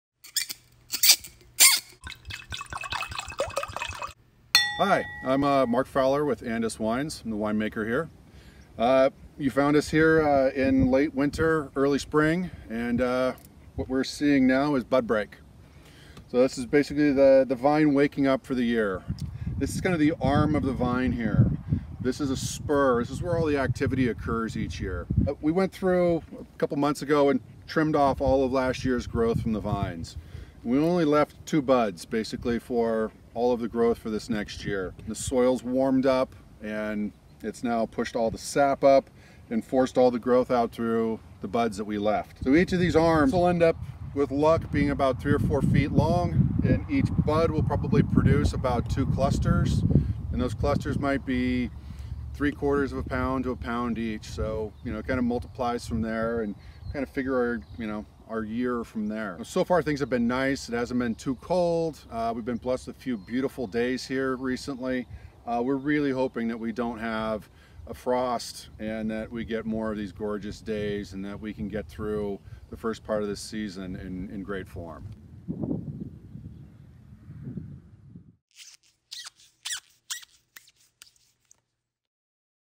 Bud Break -Spring is here, the defining season for every winemaker!
Mark Fowler, Andis Wine Maker
Spring is here, the defining season for every winemaker!  We just wrapped up winter pruning in the vineyard in February, and are now eagerly anticipating spring and bud break.
Bud break signals the beginning of this year’s vine growth. As the soil temperature rises so does root pressure, bringing once dormant buds back to life. The buds start to swell and get felty until finally the first and second leaves start to unfold.
Spring is the most nerve racking time of the year for us as a single frost can damage an entire crop. If the temperature drops below 32 degrees the delicate buds may be destroyed.  If temperatures rise too much the grapevines will grow too rapidly focusing energy on producing leaves instead of berries. Mild springs are ideal for encouraging slower growth.
If the weather is in our favor small shoots and leaves, required for photosynthesis, start to develop on the vine.  As the vine grows more buds may develop, these buds are removed to allow the vine to focus its energy on the intended growth. Eventually small flowers start blooming and tiny bunches of grapes start to grow. At this early stage we can start to determine the size of the crop and berry. Everything that happens in the spring defines the rest of the season!
Follow along with our 2020 harvest as we update our blog and social media all season long.